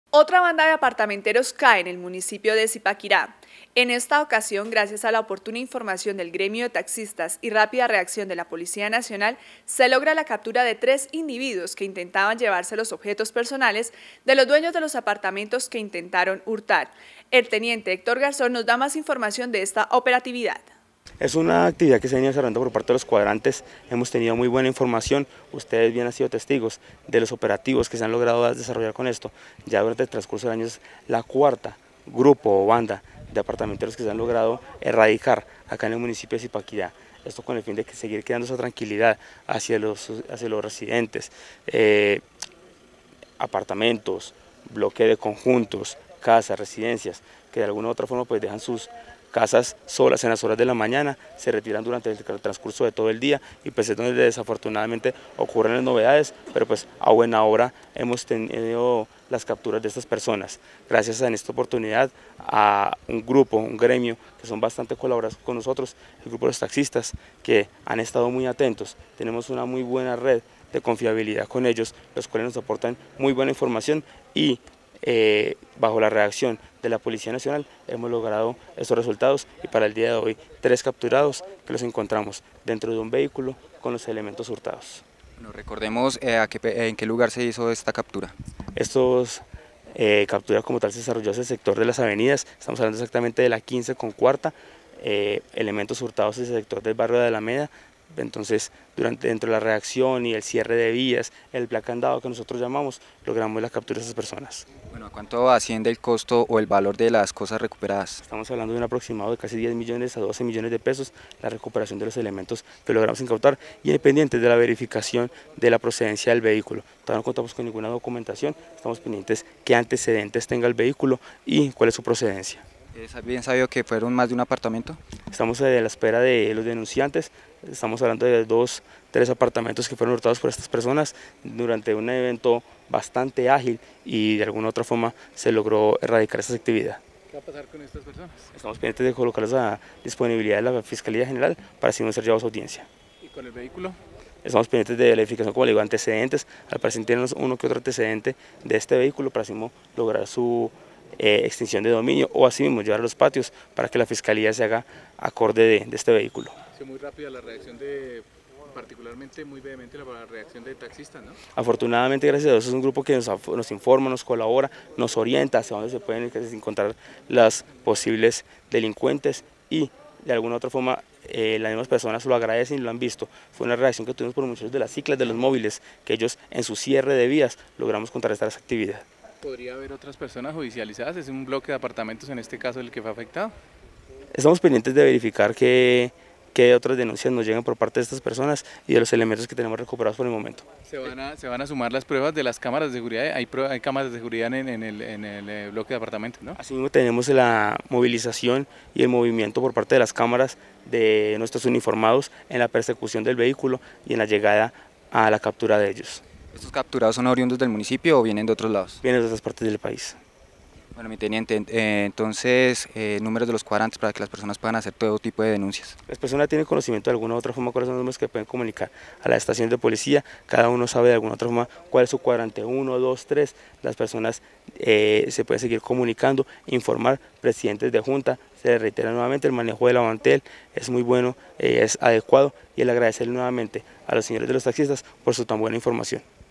CAPTURADA BANDA DE APARTAMENTEROS EN ZIPAQUIRÁ
El día 24 de marzo, gracias al aviso oportuno del gremio de taxistas a la policía se logró capturar a esta banda que se desplazaba en un vehículo con las pertenencias hurtadas a los habitantes de los apartamentos, recuperando elementos equivalentes a 12 millones de pesos aproximadamente.